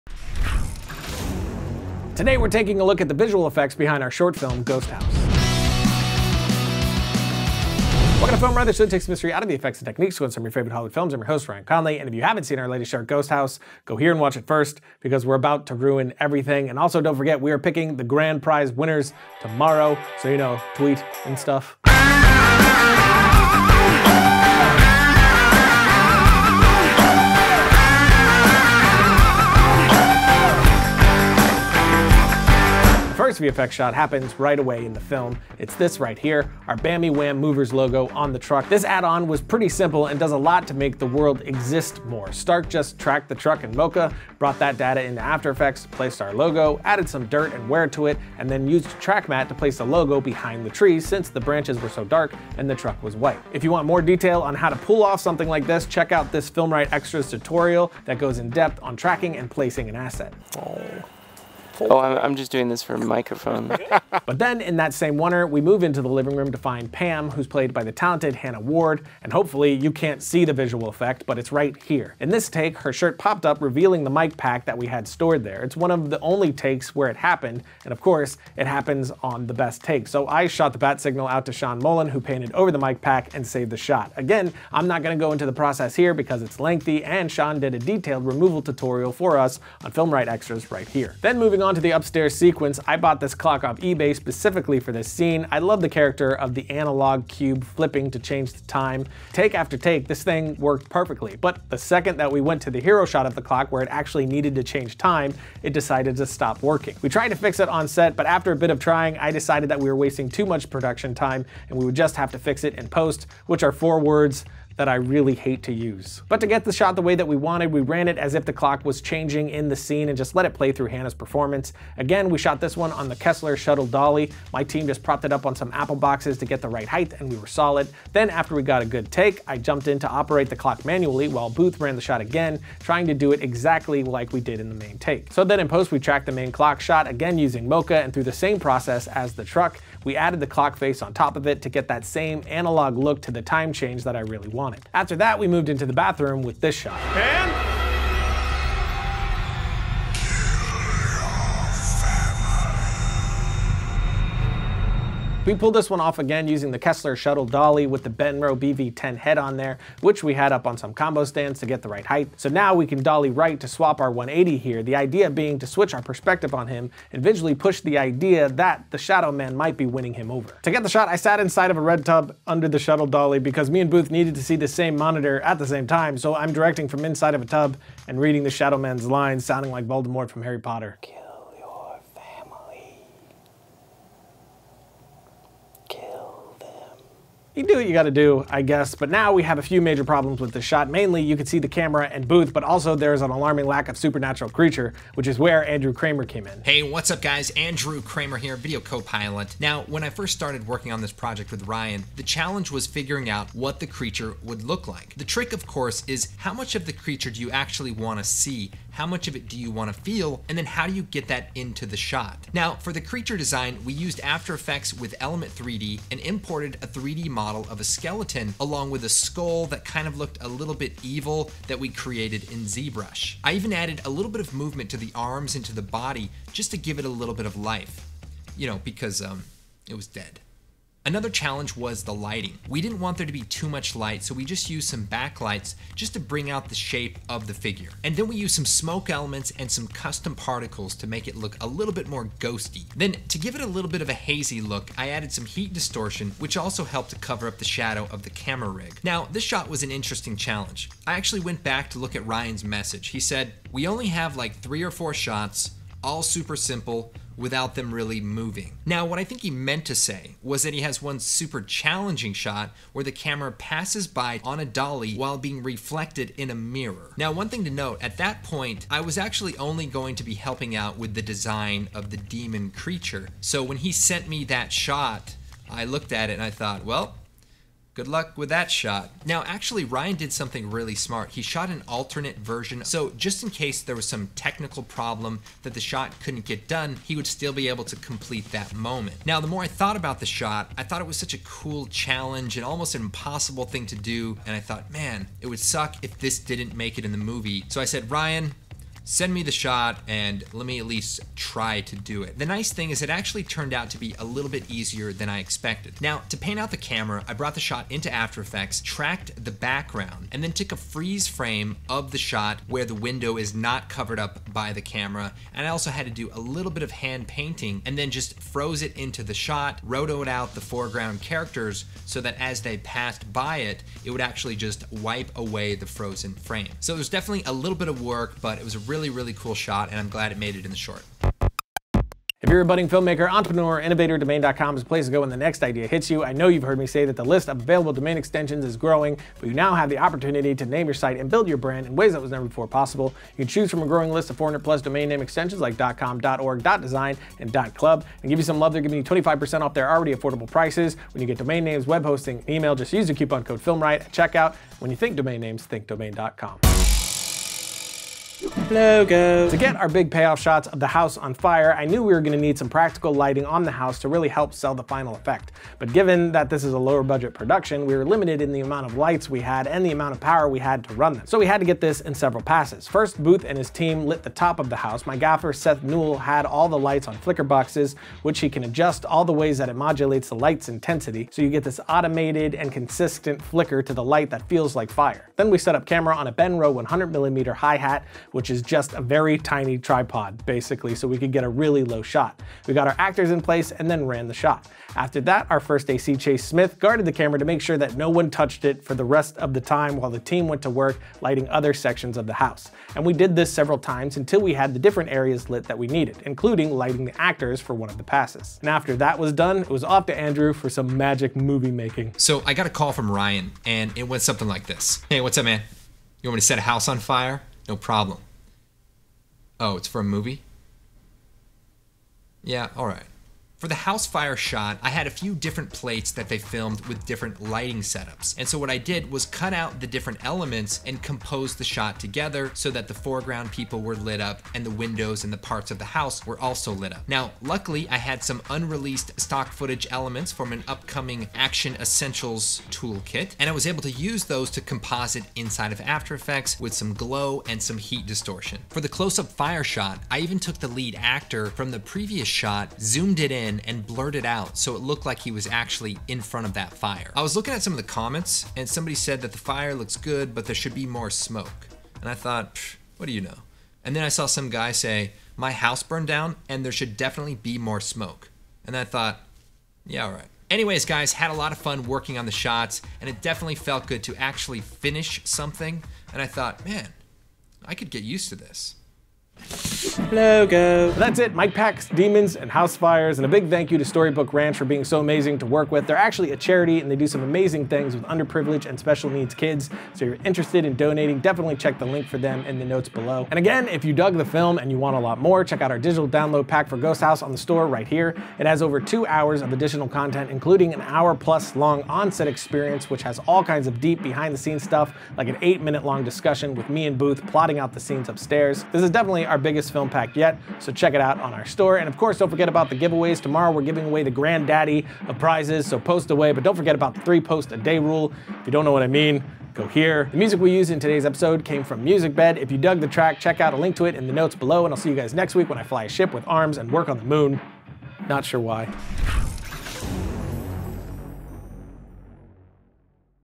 Making a Monster & Setting a House On Fire
Today, Ryan and Andrew Kramer talk the VFX of Ghost House!

Get Ghost House with over with Almost Three Hours of Special Features